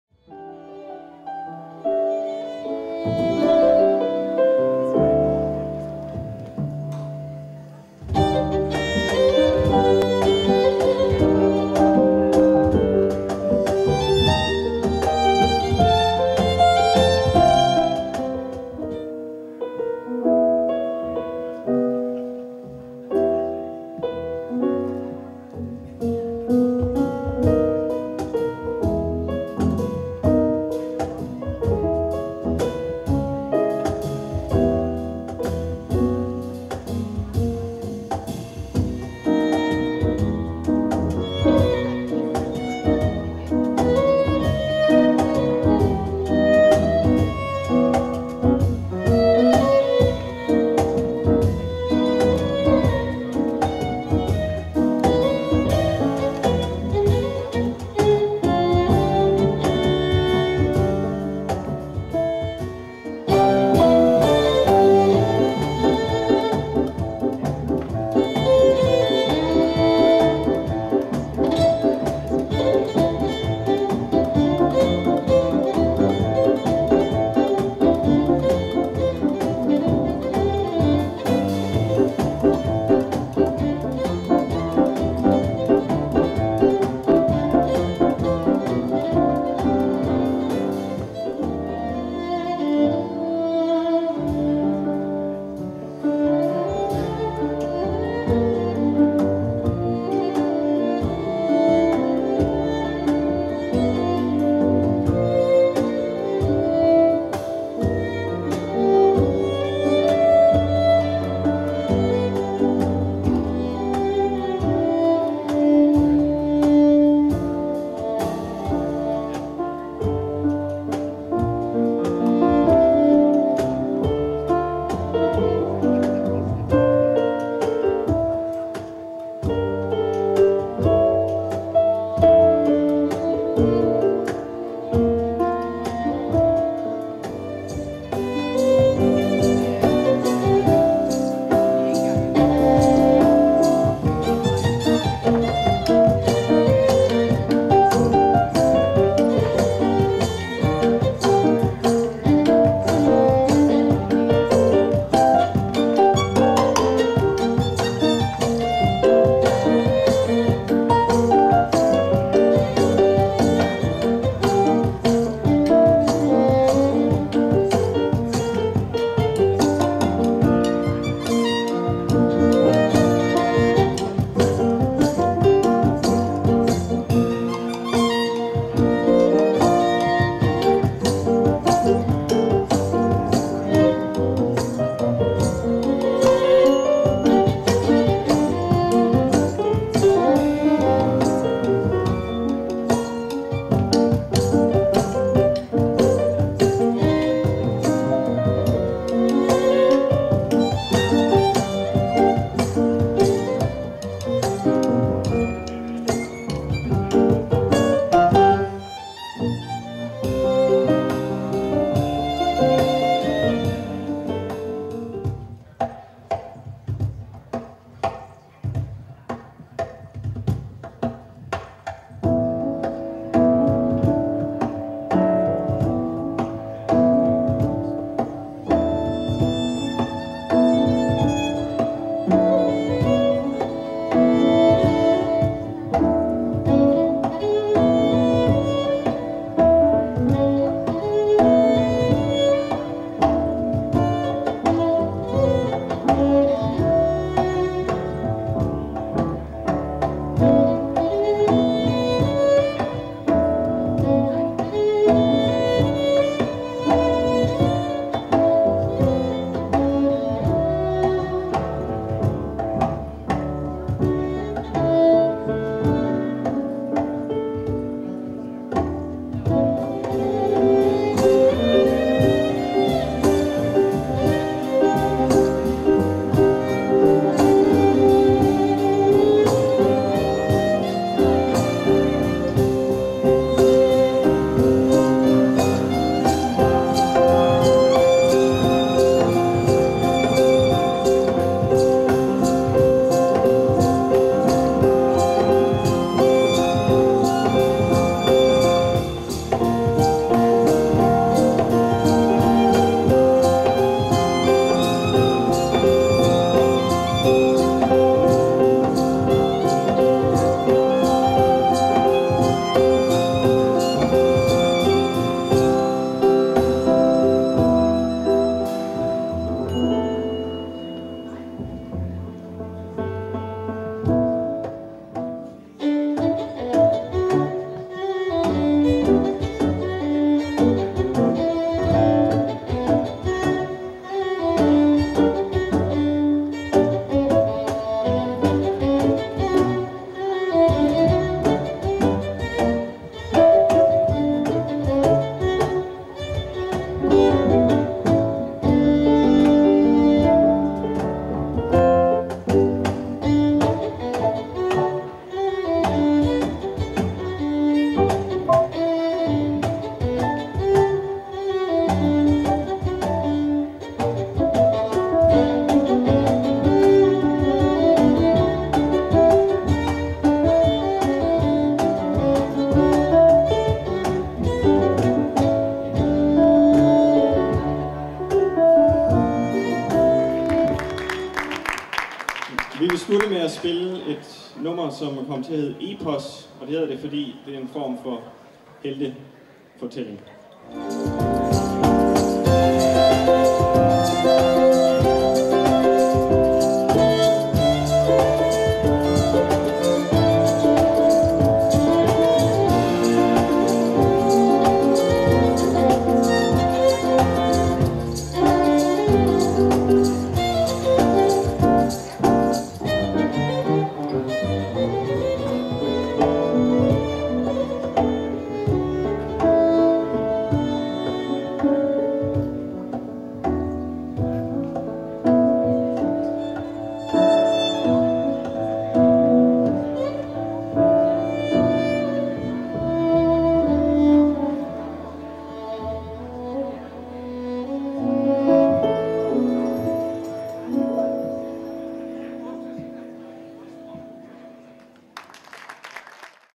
TRÆVÆRK - highlights at Halvøens Vinterjazz 2019
From our debut concert at Halvøens Vinterjazz, Kolding 2019.

TRÆVÆRK is new Nordic instrumental music that operates between rhythmic and classical genres. For love of melody and harmony, the music is presented in a danceable and open folk tone.

Jacob Agerskov Buur - violin
Asger Agerskov Buur - piano
Mathilde Qvist - bass
Dea Marie Kjeldsen - percussion